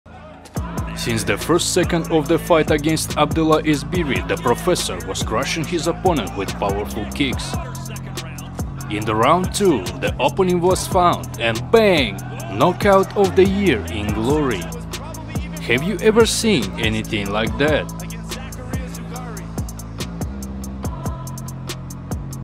KO of The Year in Glory Kickboxing, Petchpanomrung Knockout
Knockout of The Year in Glory Kickboxing, Petchpanomrung Kiatmukao vs Abdellah Ezbiri